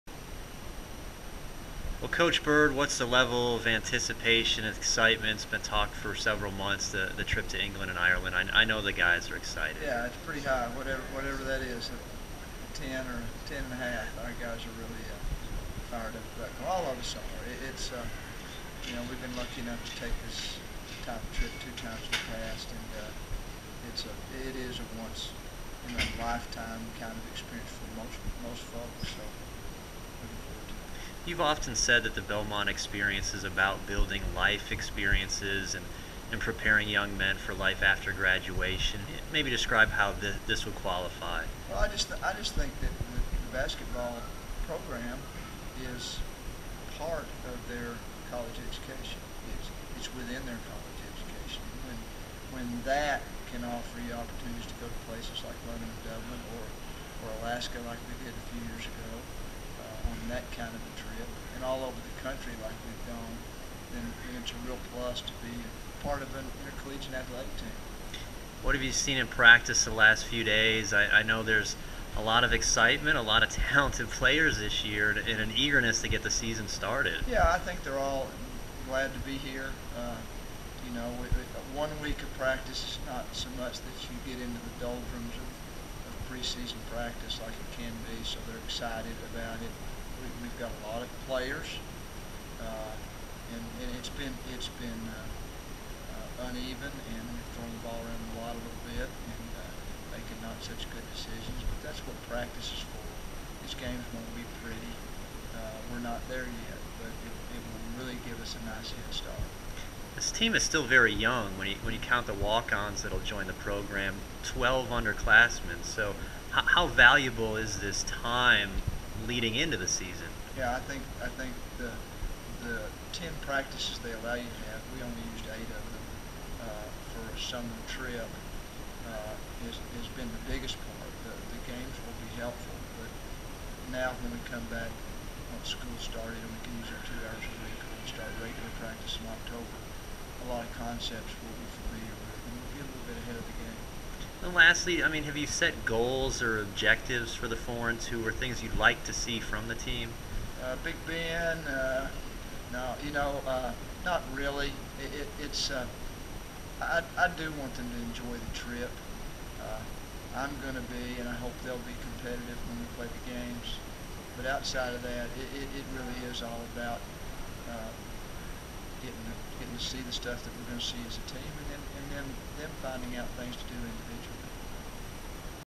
Belmont Basketball 2015 Foreign Tour Preview
Belmont men's basketball head coach Rick Byrd previews the Bruins' foreign tour of England and Ireland